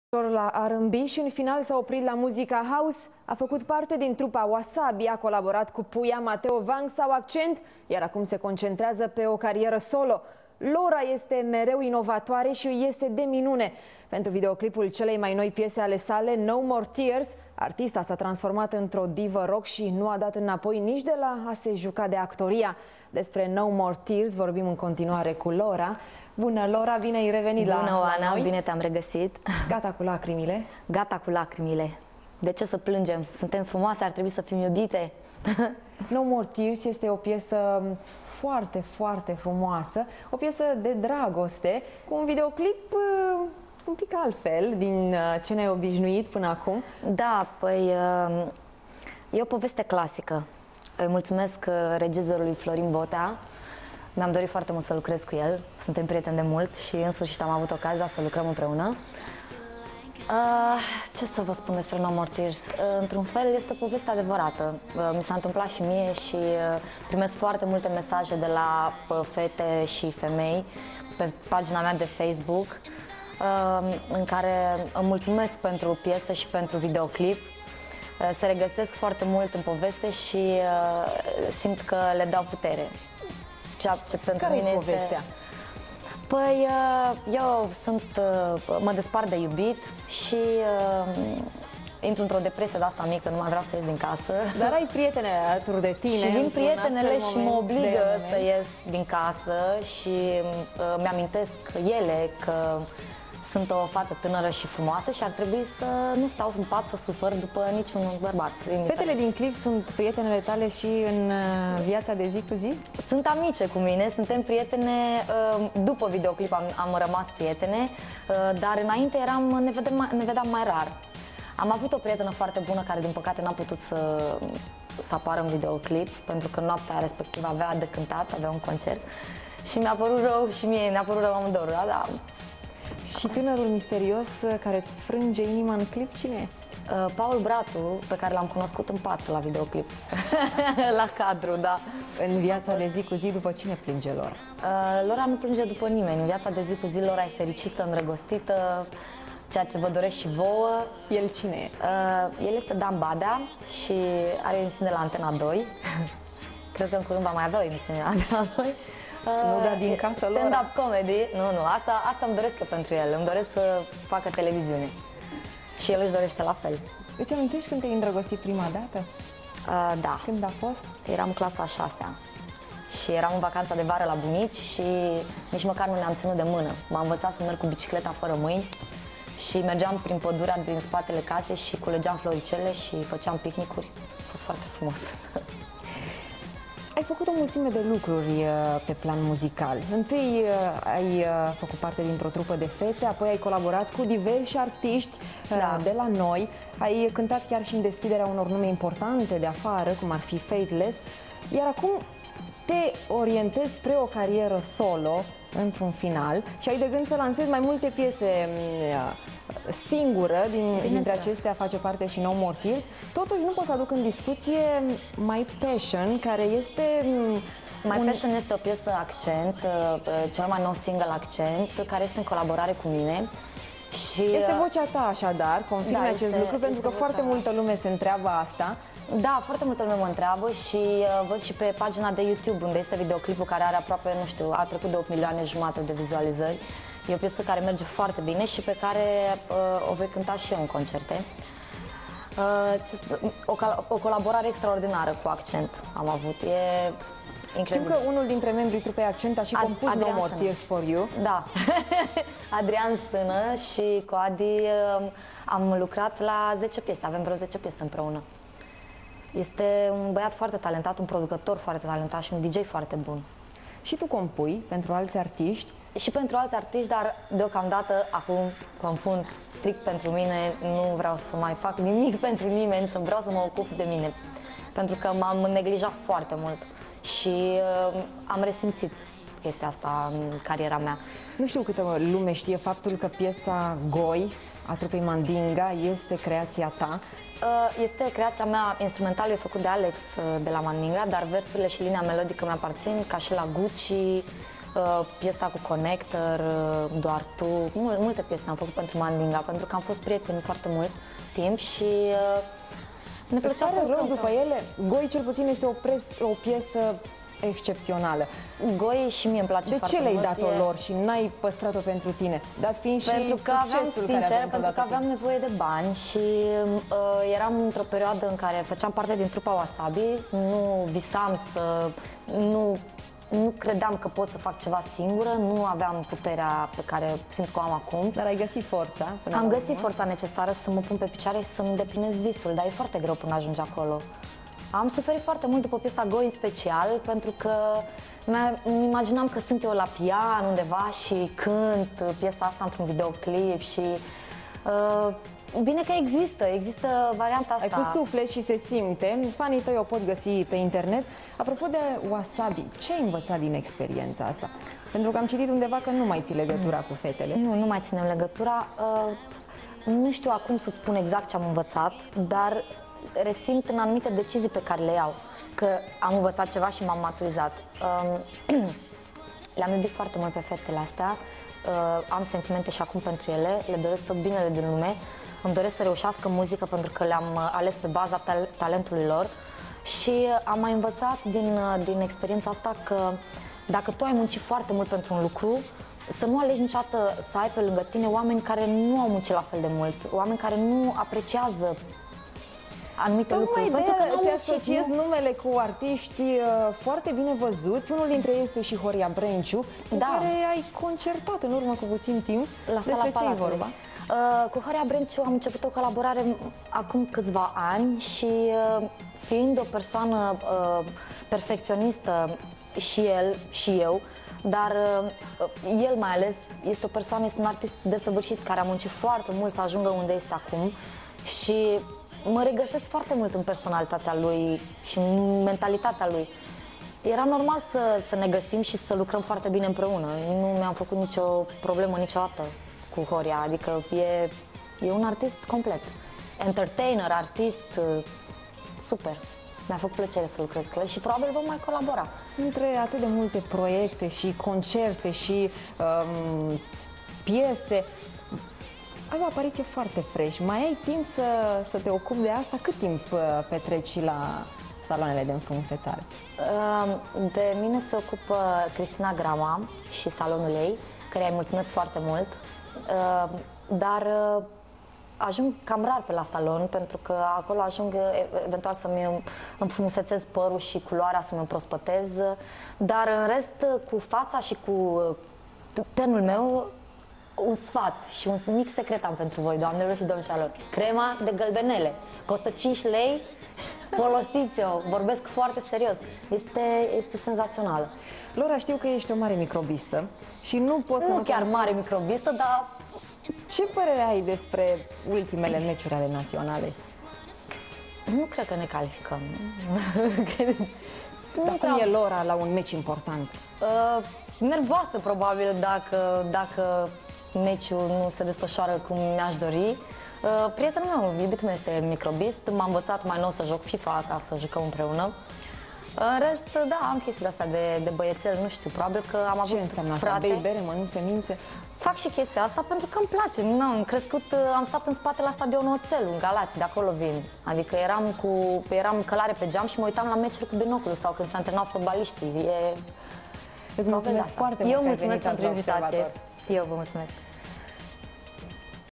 Lora - un nou stil un nou look 1 APRILIE 2011
Mai intai au fost baladele apoi a trecut usor la R-ampampB si in final s-a oprit la muzica house. A facut parte din trupa Wassabi a colaborat cu Puya Matteo Vunk sau Akcent iar acum se concentreaza pe o cariera solo. Lora este mereu...